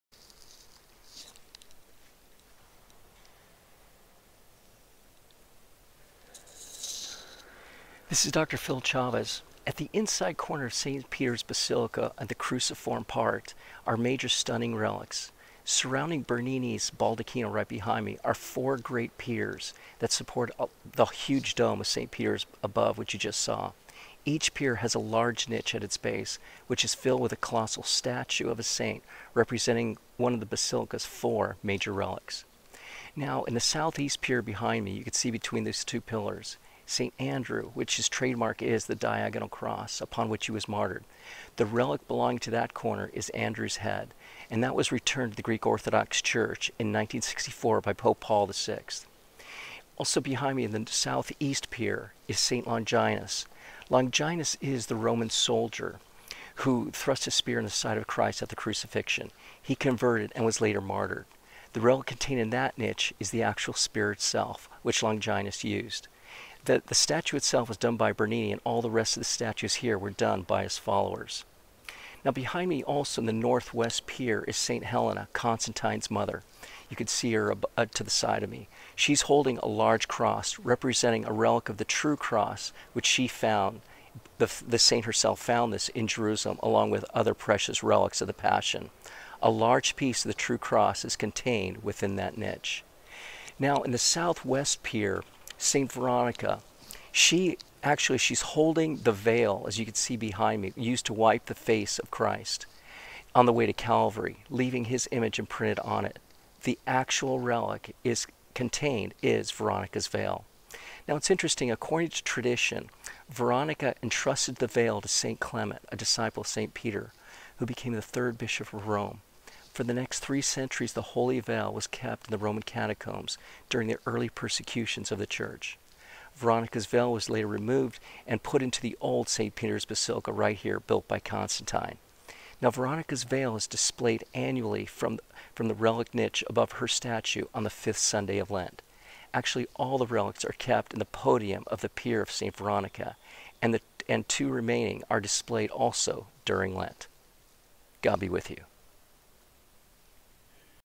FOUR PIER RELICS
Four great piers support the dome of St. Peter's Basilica. Each pier niche holds the basilica's four major relics.